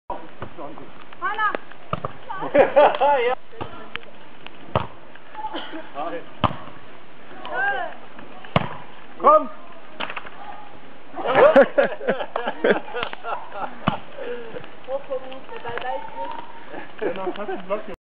Spug Ausflug Beachvolleyball Illoshöhe Osnabrück 27.August 2009. Spaß und Geselligkeit
Spug.Ausflug. Beachvolleyball.Illoshöhe.Osnabrück.27.August.2009.
Prokrastinationsvorschlag des sonnigen, frühen Abends: Beachen!
Steht die Sonne nicht mehr hoch am Himmel, Brennt die Sonne nicht mehr so heiß, weht ein laues Lüftchen und deine tägliche Dosis Geselligkeit steht an? Trefft euch zum Beachvolleyball.
Es gibt zahlreiche Plätze in Osnabrück, auf denen man Strandvolleyball spielen kann, die meisten sind frei zugänglich und benetzt/vernetzt/aufgetakelt/bespannt. Also los! Dieses Mal Illoshöhe